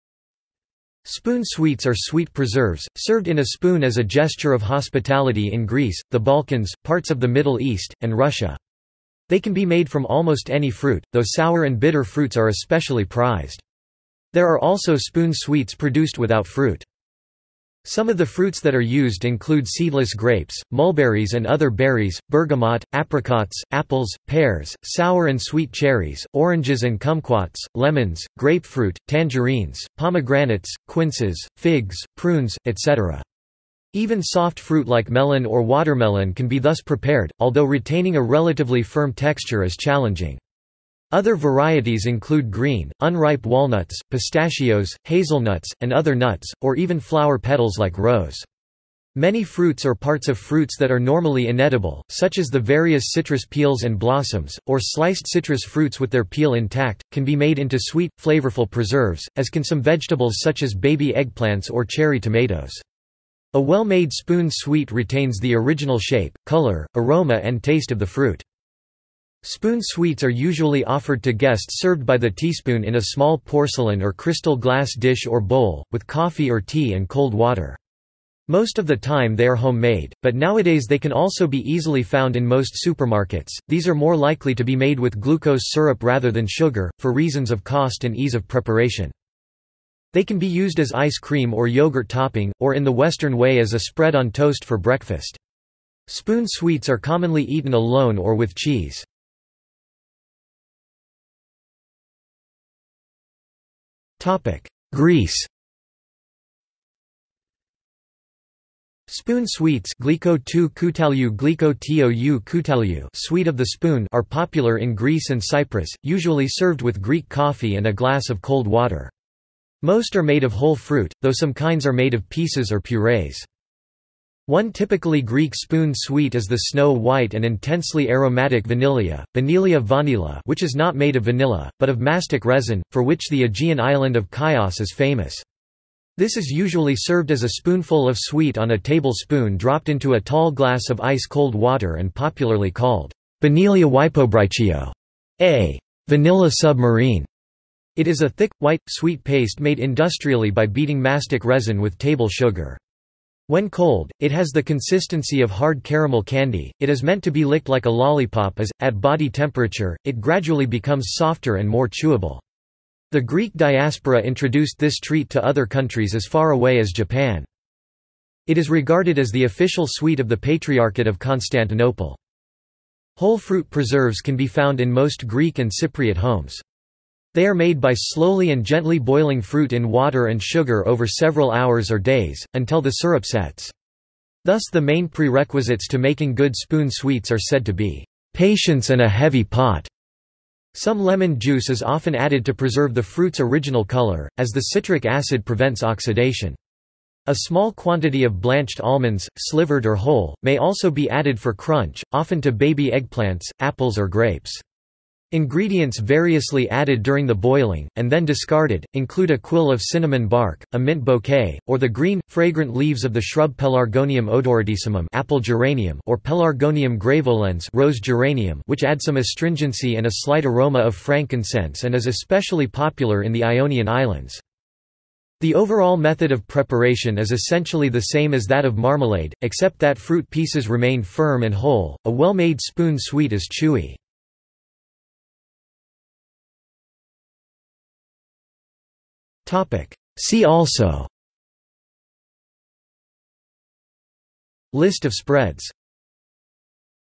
Spoon sweets | Wikipedia audio article
This is an audio version of the Wikipedia Article:
Spoon sweets

00:01:40 1 Greece
00:03:55 2 See also

- Socrates

SUMMARY
Spoon sweets are sweet preserves, served in a spoon as a gesture of hospitality in Greece, the Balkans, parts of the Middle East, and Russia. They can be made from almost any fruit, though sour and bitter fruits are especially prized. There are also spoon sweets produced without fruit.
Some of the fruits that are used include seedless grapes, mulberries and other berries, bergamot, apricots, apples, pears, sour and sweet cherries, oranges and kumquats, lemons, grapefruit, tangerines, pomegranates, quinces, figs, prunes, etc.  Even soft fruit like melon or watermelon can be thus prepared, although retaining a relatively firm texture is challenging.  Other varieties include green, unripe walnuts, pistachios, hazelnuts, and other nuts, or even flower petals like rose. Many fruits or parts of fruits that are normally inedible, such as the various citrus peels and blossoms, or sliced citrus fruits with their peel intact, can be made into sweet, flavorful preserves, as can some vegetables such as baby eggplants or cherry tomatoes. A well-made spoon sweet retains the original shape, color, aroma and taste of the fruit.
Spoon sweets are usually offered to guests served by the teaspoon in a small porcelain or crystal glass dish or bowl, with coffee or tea and cold water.  Most of the time they are homemade, but nowadays they can also be easily found in most supermarkets; these are more likely to be made with glucose syrup rather than sugar, for reasons of cost and ease of preparation.
They can be used as ice cream or yogurt topping, or in the Western way as a spread on toast for breakfast. Spoon sweets are commonly eaten alone or with cheese.